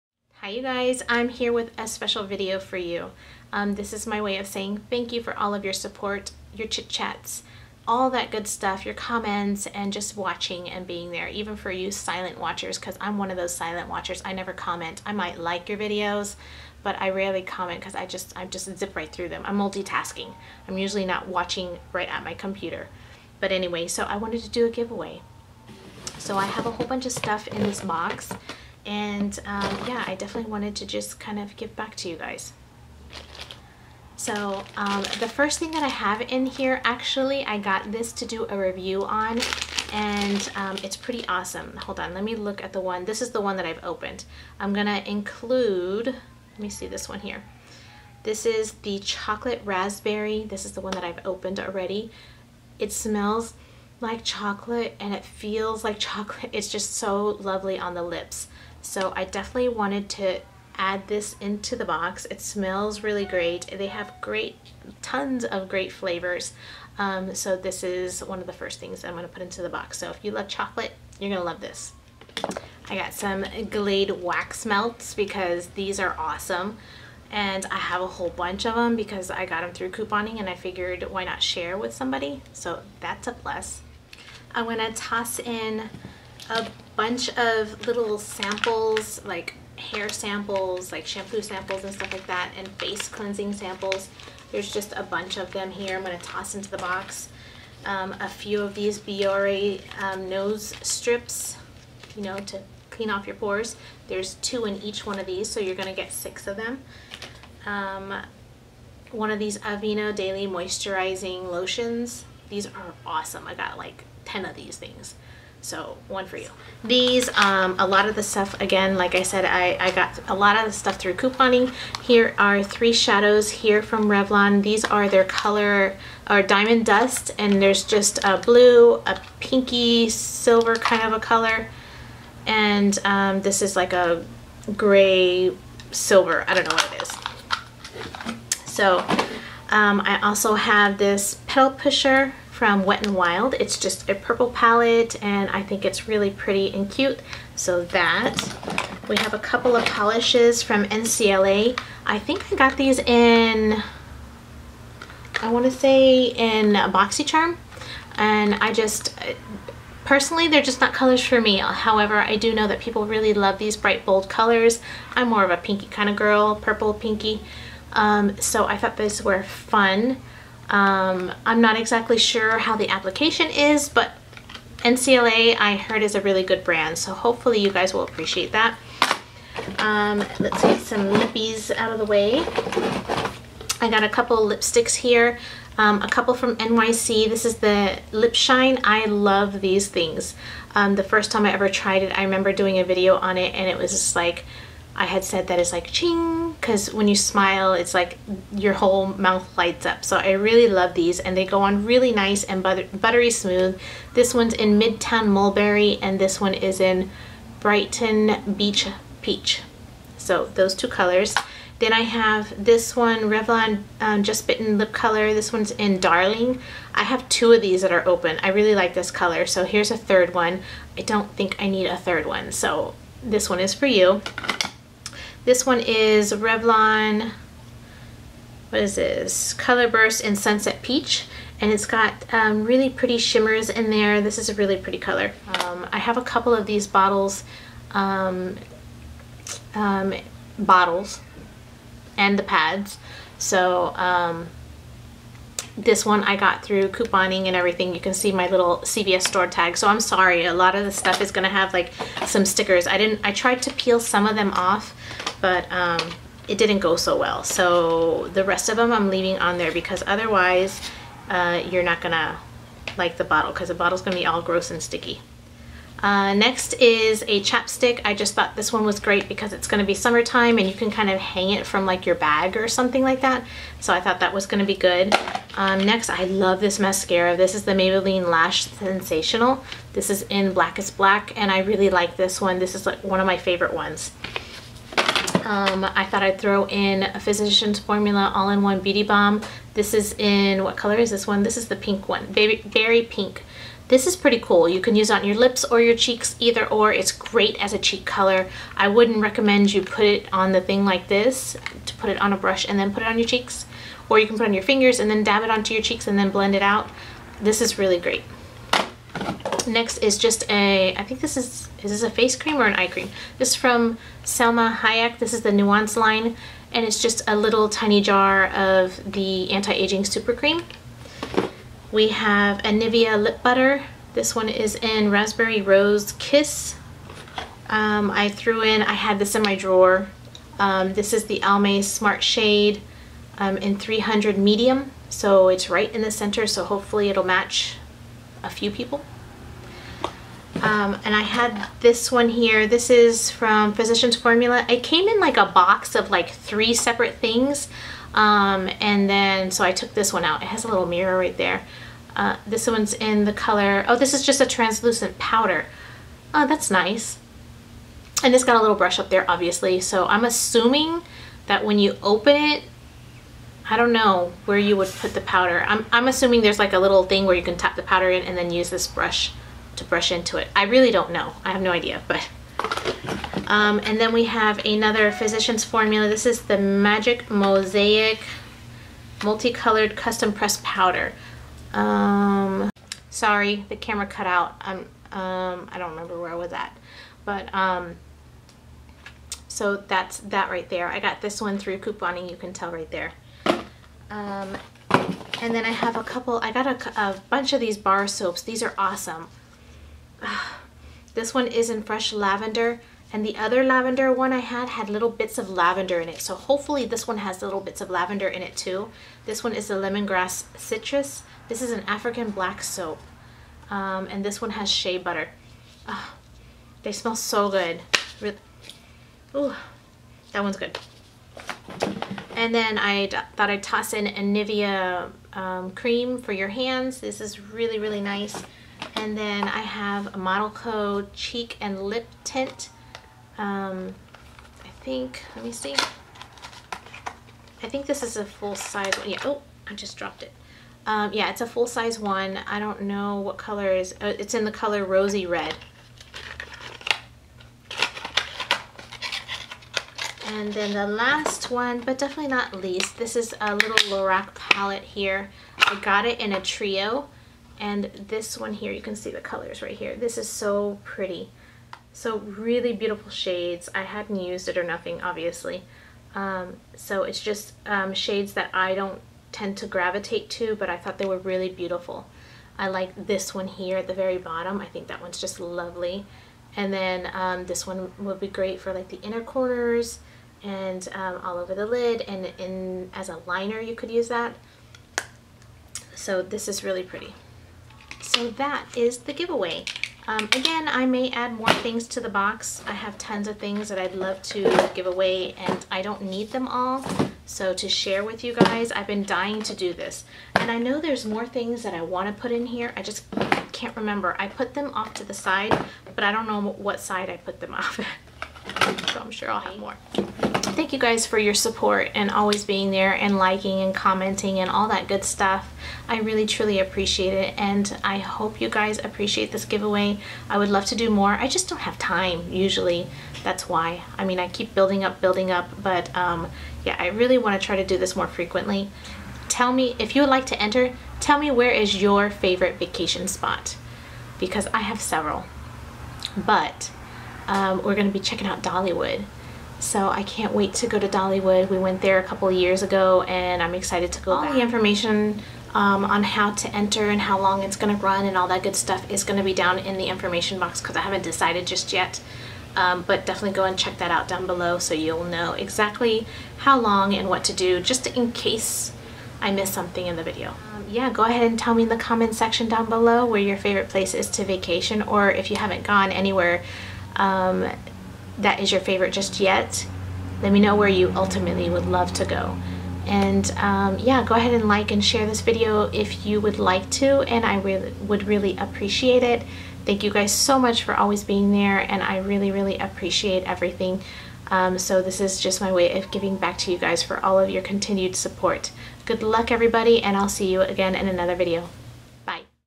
Summer Mixed Box Giveaway **CLOSED**
Winner is Wendy! pls be sure to respond to my message within 48 hours.
This giveaway is a way for me to share the amazing things I love or have tried and have extras for you to try out too. I guess since I love to coupon, review and more I get opportunities for me and for you.
The giveaway run for the month of June so you have plenty of times to enter. Answer the question I asked in the video to enter.  you can enter once daily with a new comment. The more you come back to enter the better your chances are. I truly appreciate all of your support!

Here are a few of the items in the box
Lorac eye shadow Quad
Maybelline mascara
GANACHE FOR LIPS ALL-NATURAL LIP BALM in Chocolate Raspberry

(choose top left drop down menu for your country)